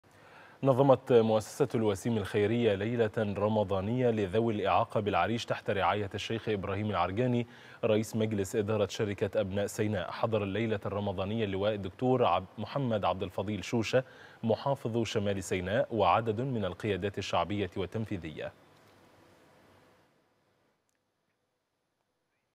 غرفة الاخبار| مؤسسة الوسيم الخيرية تنظم ليلة رمضانية لذوي الإعاقة بالعريش
6 أبريل 2023